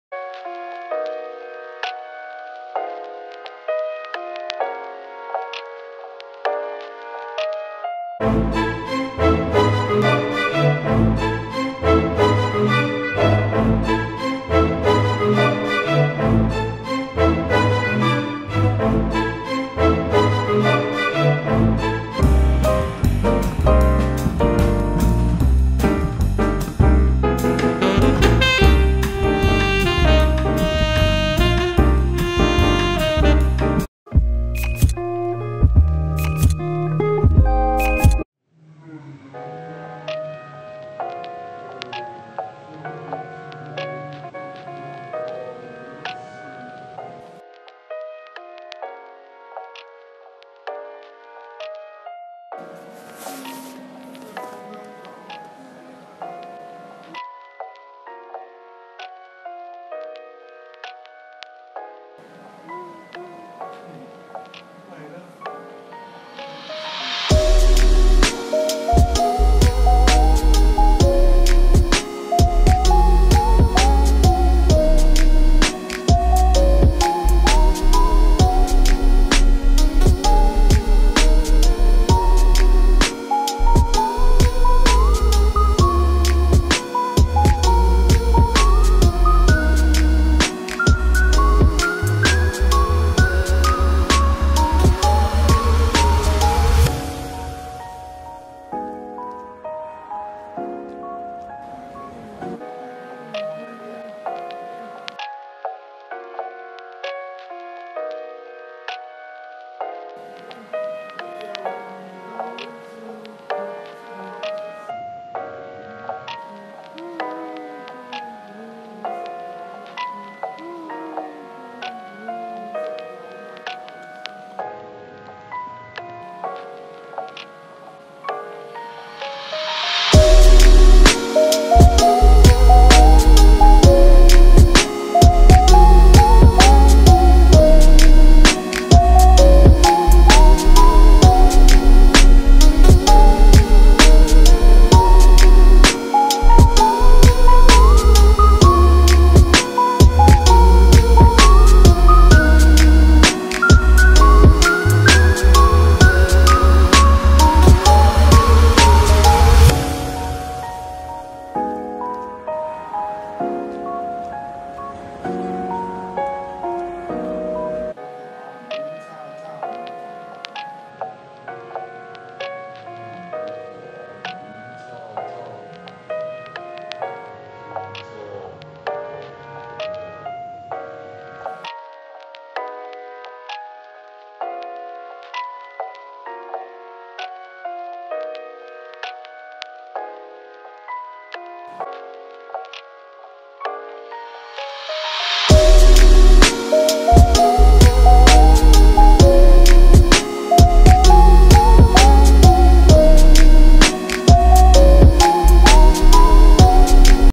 CORGI falls asleep while SUN TANNING✰petting my corgi
HOPE THIS VIDEO CHEERED UP YOUR DAY.
The background music is meant to be relaxing and chill so sit back and watch!
My corgi is sun tanning outside on the balcony and enjoying life. I pet him to sleep, and he sleeps very very peaceful! He is sleeping outside on the balcony and is the cutest corgi you will ever see!
gooodbyee✌️